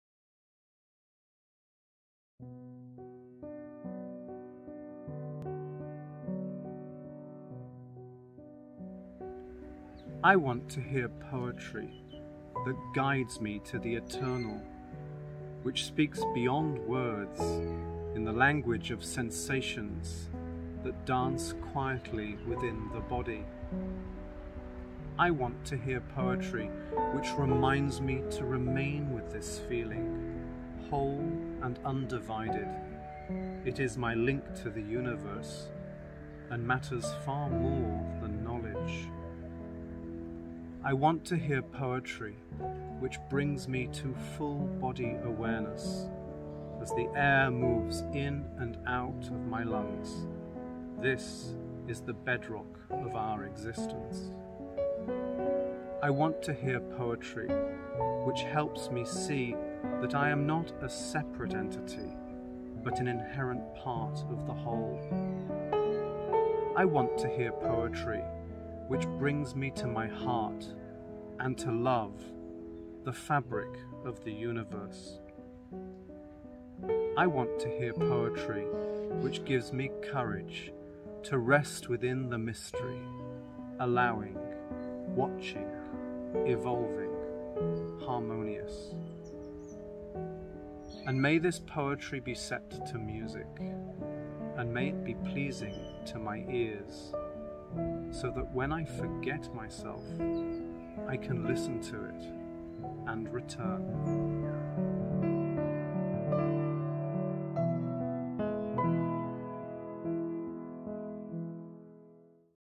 The Feeling of Being Alive
I want to hear poetry
That guides me to the eternal
Which speaks beyond words
In the language of sensations
That dance quietly within my body.

I want to hear poetry which
Reminds me to remain with this feeling
Whole and undivided
It is my link to the universe
And matters far more than knowledge.

Which brings me
To full body awareness
As the air moves in and out
Of my lungs
This is the bedrock of our existence.

Which helps me see that
I am not a separate entity
But an inherent part of the whole.

Which brings me to my heart
And to love
The fabric of the universe.

Which gives me courage
To rest within the mystery
Allowing, watching, evolving,
Harmonious.

And may this poetry be set to music,
And may it be pleasing to my ears,
So that when I forget myself,
I can listen to it and return.